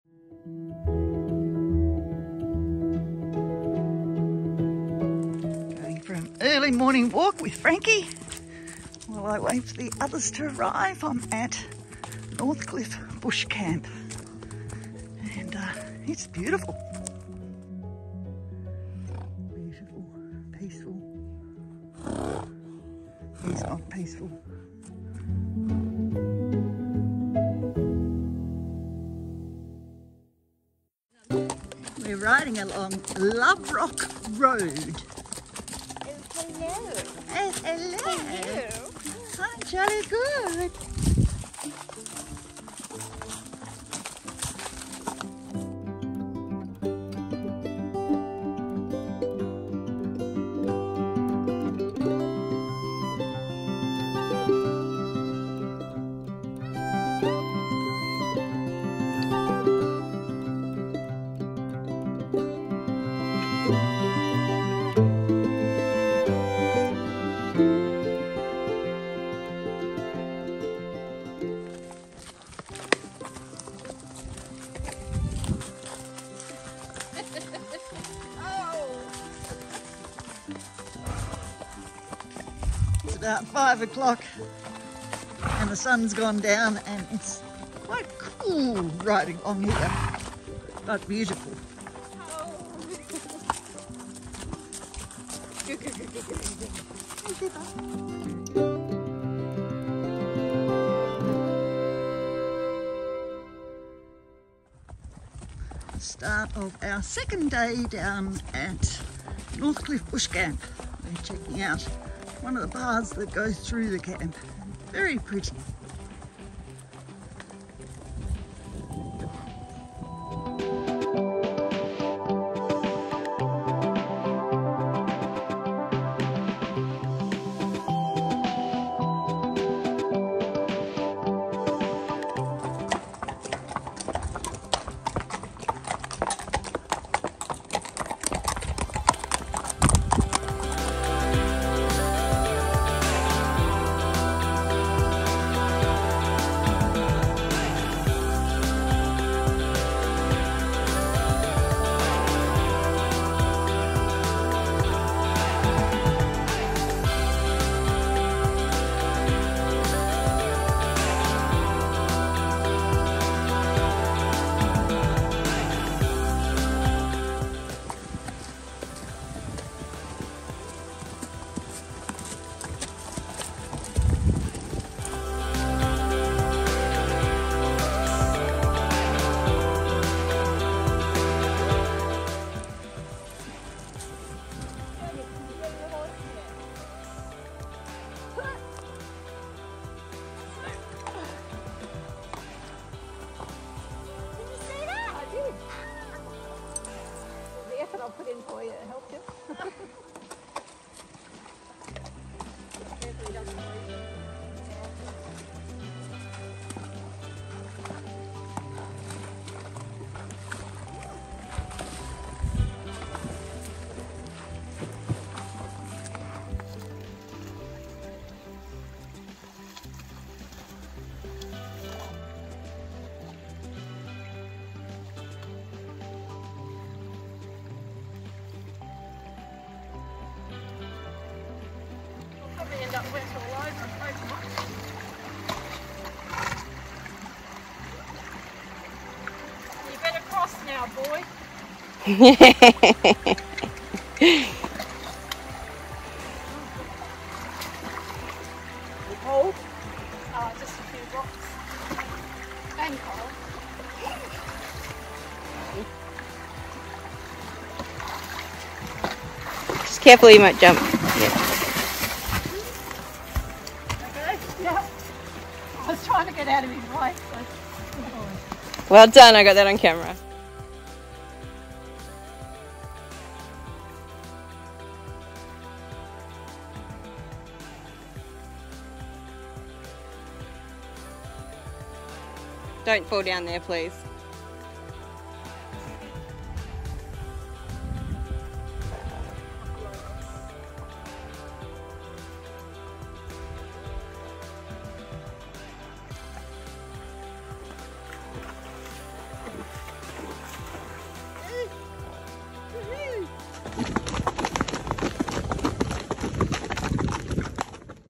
Exploring Northcliffe
We stayed at Northcliffe Bush Camp with our horses and explored the local trails, topped off with lunch at the Northcliffe Hotel.